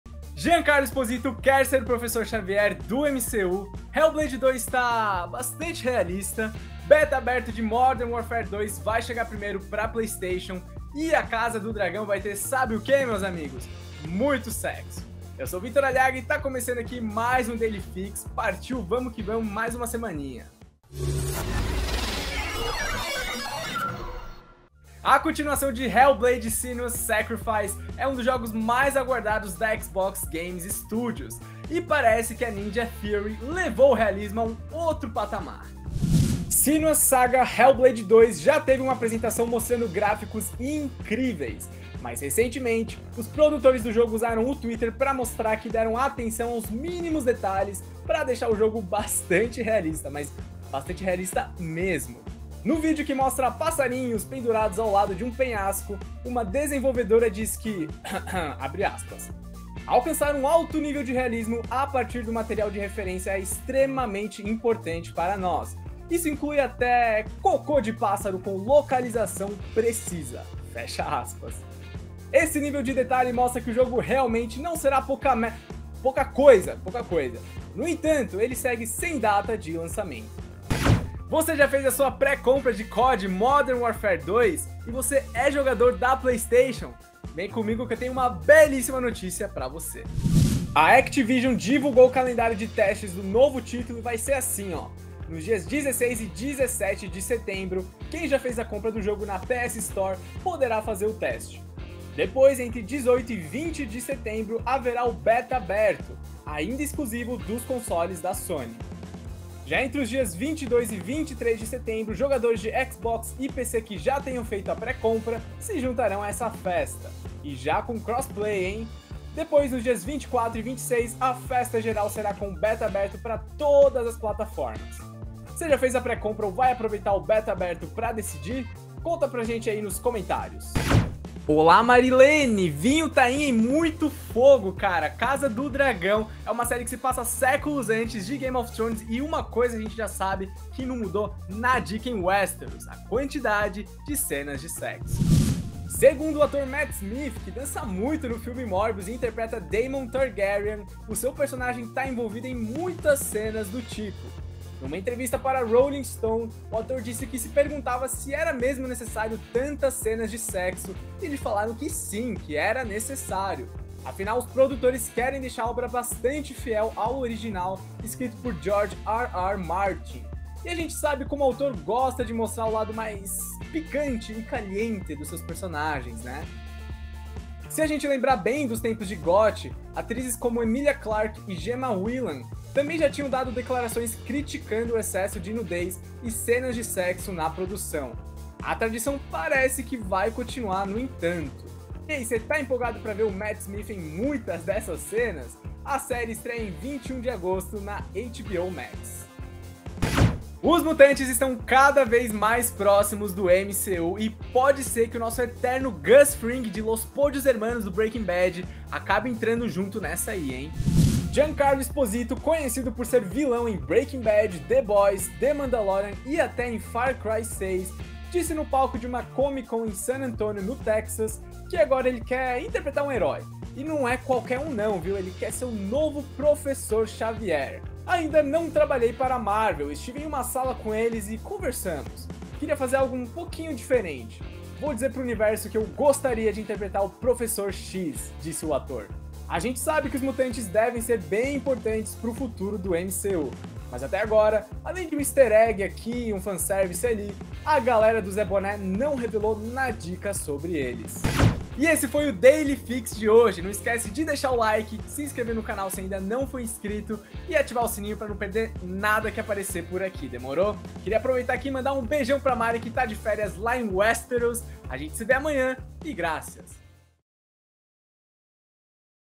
MATT SMITH DIZ QUE HOUSE OF THE DRAGON TEM MUITAS CENAS PICANTES - Daily Fix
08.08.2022 - Senua's Saga: Hellblade 2 está mais realista do que pensávamos; Beta aberto de Call of Duty Modern Warfare 2 chega primeiro ao PlayStation; Giancarlo Esposito quer ser o novo Professor Xavier; Matt Smith diz que spin off de Game of Thrones tem muitas cenas daquilo…

ACOMPANHE O IGN BRASIL!